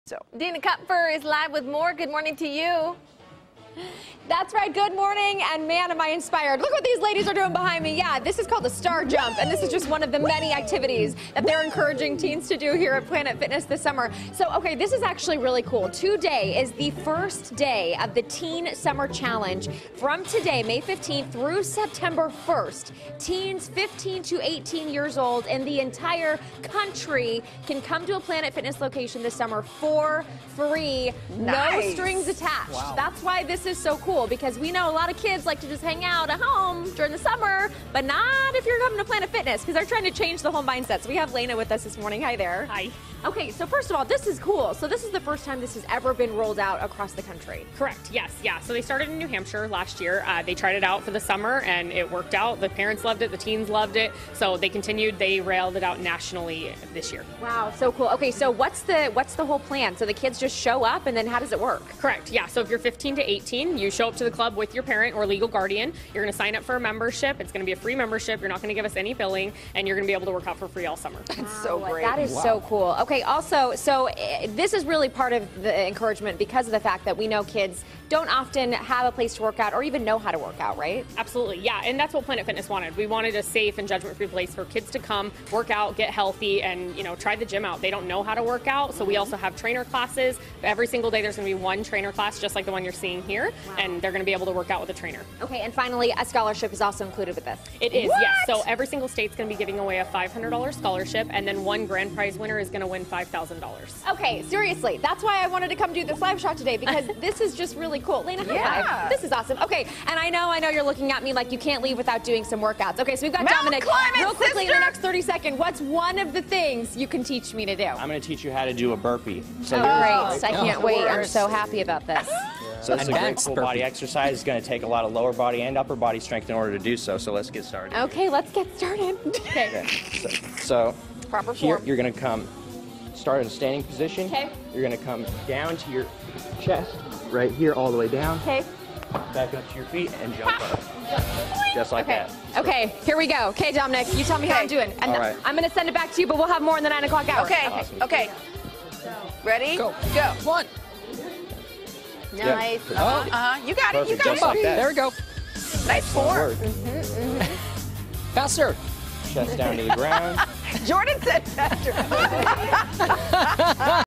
Teen Summer Challenge
Planet Fitness is letting teens work out in their gyms for free if they sign up for a fitness challenge!